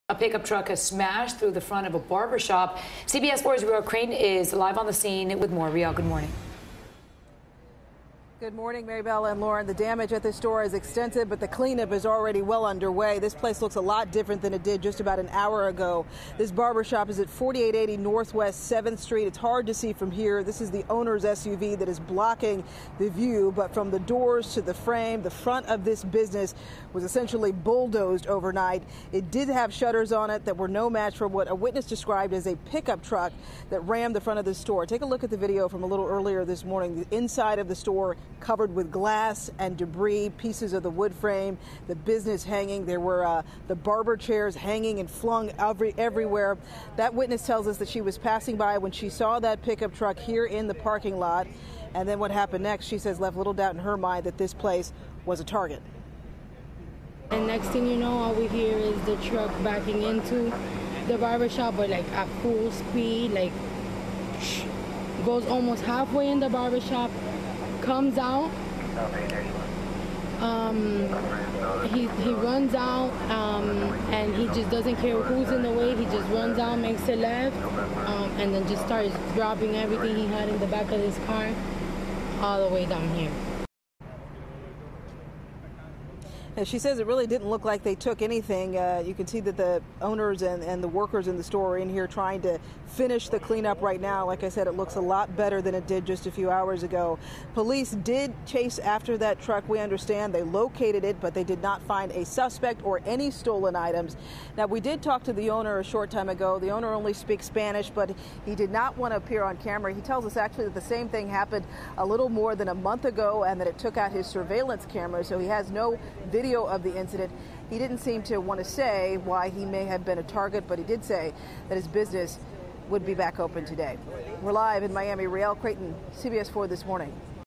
Pick Up Truck Smashed Into Miami Barber Shop
Police found the truck, but not the driver or any of the stolen items